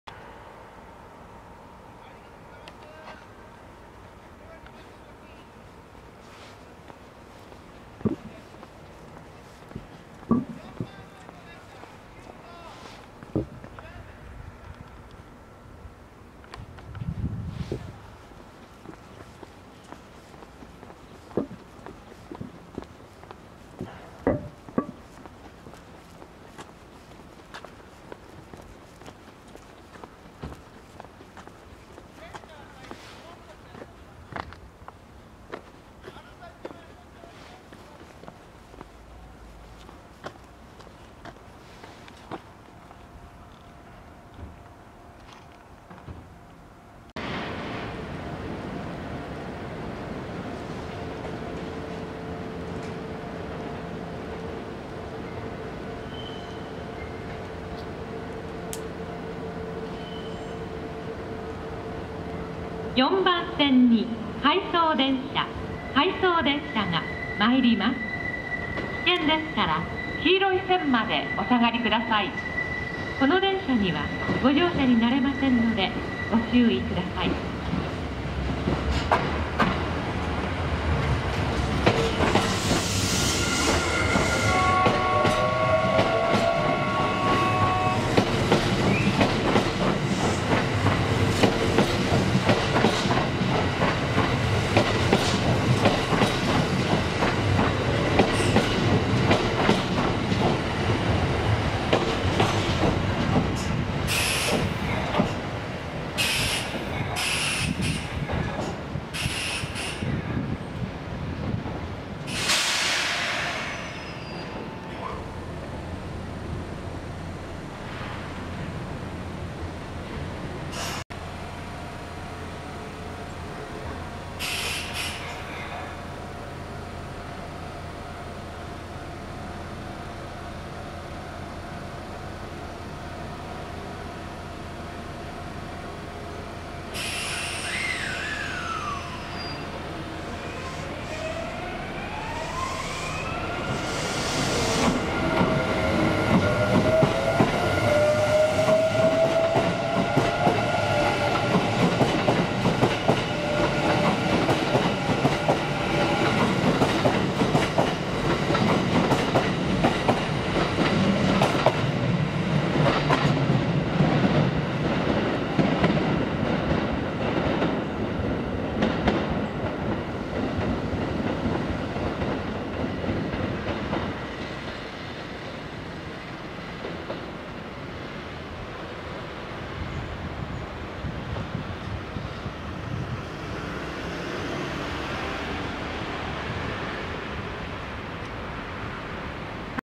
近鉄5820系DF51（シリーズ21 L/Cカー） 車輪交換出場回送
大阪線で運用されている高安検車区所属の5820系DF51（C#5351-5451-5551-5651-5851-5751　シリーズ21・Ｌ/Ｃカー）が、五位堂検修車庫でC#5351・C#5651・C#5851・C#5751の車輪交換を終えて出場する動画です。
00:00　五位堂検修車庫内留置中（敷地外より撮影）
00:47　五位堂検修車庫出庫～五位堂駅4番線に入線
02:00　五位堂駅4番線発車